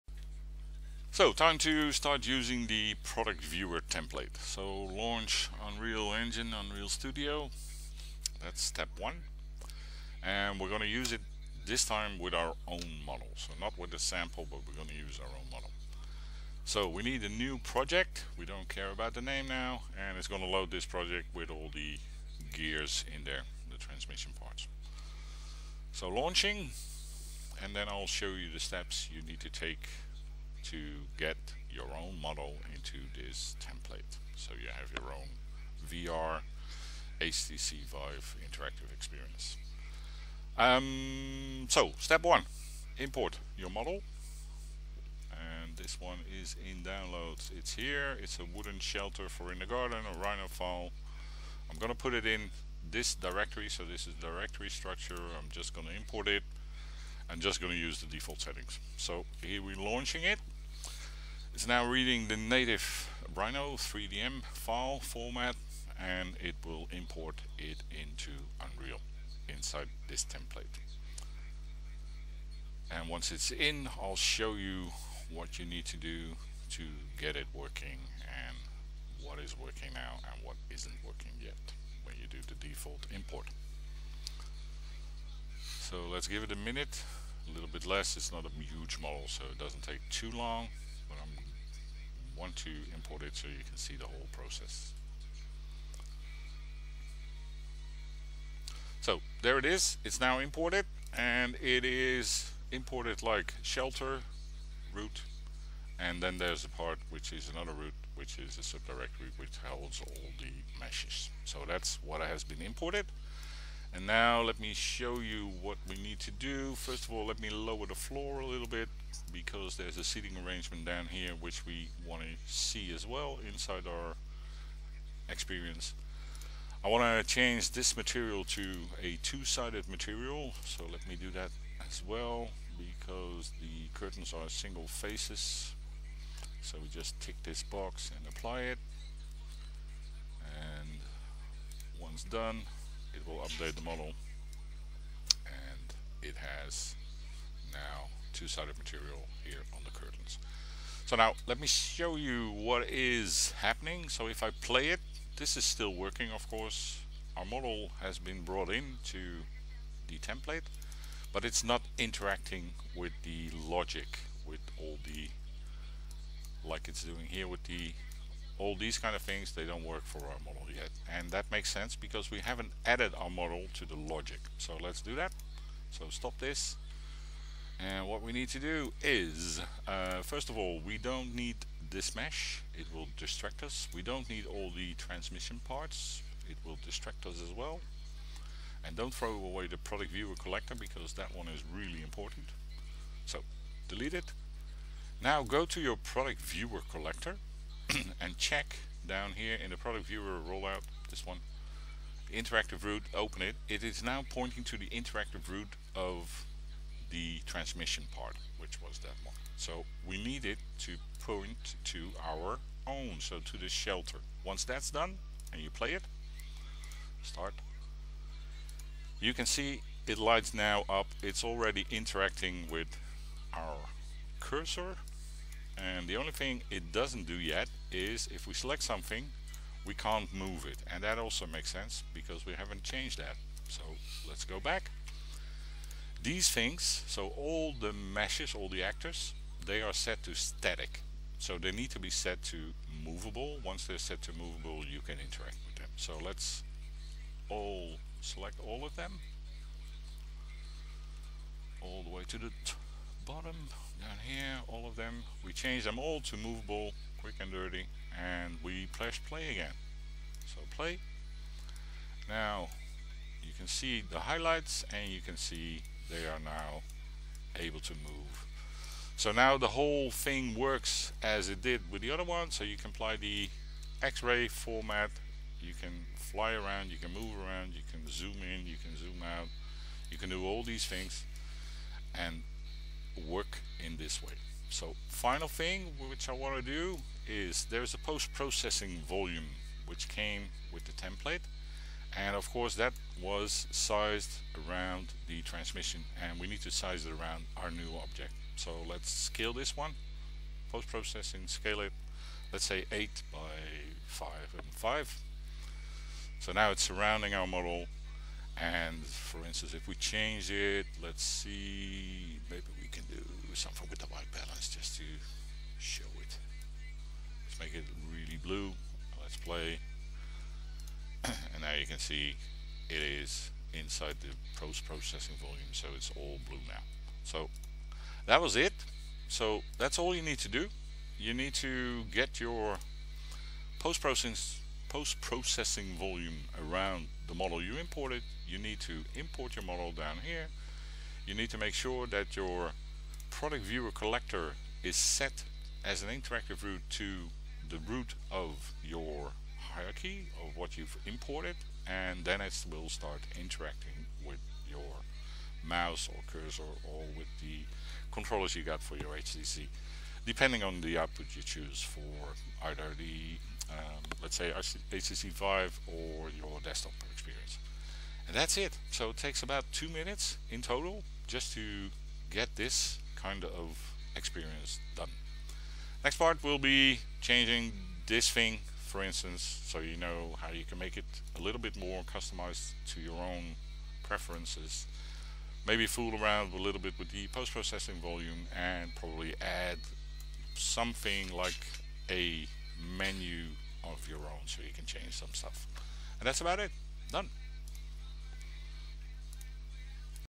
Unreal Studio Productviewer Template, Part 2
Using the UnrealStudio Product Viewer Template with your own model and the steps you need to take to create an interactive experience in less than 2 minutes